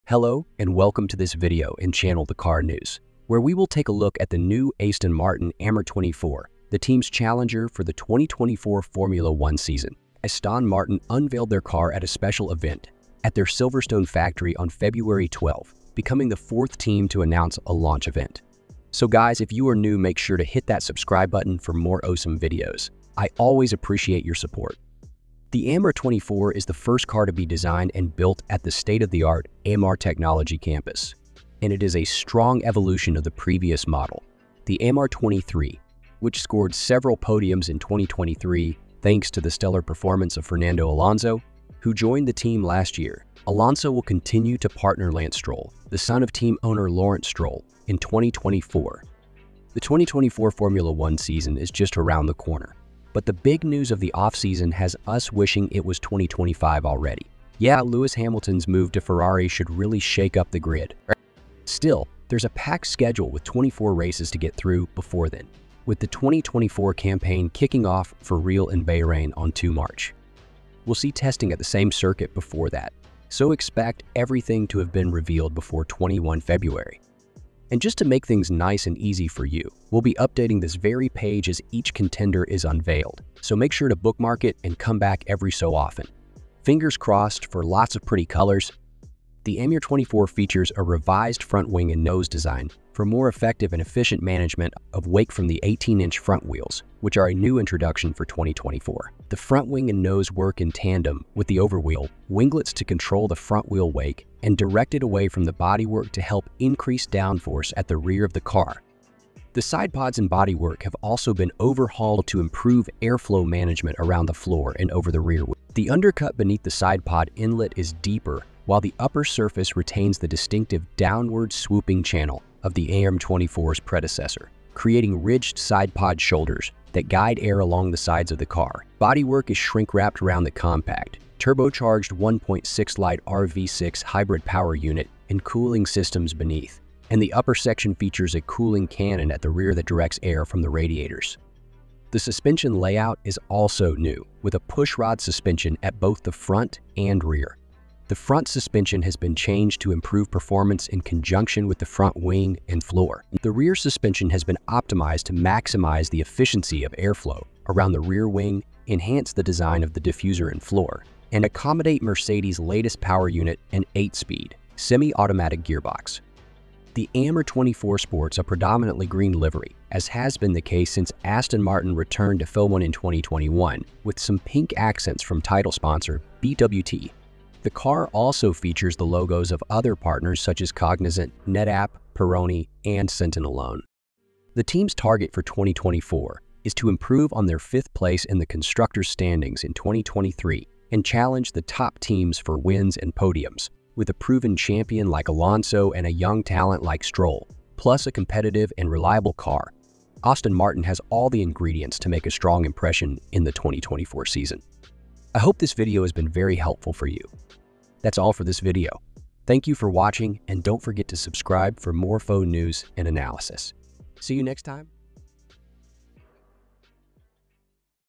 F1 cars 2024: Aston Martin reveals the AMR24 | All-New 2024 Aston Martin AMR24 Has Been Launched
In this video, we will take a look at the new Aston Martin AMR24, the team's challenger for the 2024 Formula 1 season. The AMR24 is the first car to be designed and built at the state-of-the-art AMR Technology Campus, and it features a revised front wing and nose design, a push-rod suspension system, and a new supercharged V8 engine. The car also sports a predominantly green livery with some pink accents from title sponsor BWT. The team's target for 2024 is to improve on their fifth place in the constructors' standings in 2023 and challenge the top teams for wins and podiums. With a proven champion like Fernando Alonso and a young talent like Lance Stroll, plus a competitive and reliable car, Aston Martin has all the ingredients to make a strong impression in the 2024 season. Watch the video to find out more about the AMR24 and its features.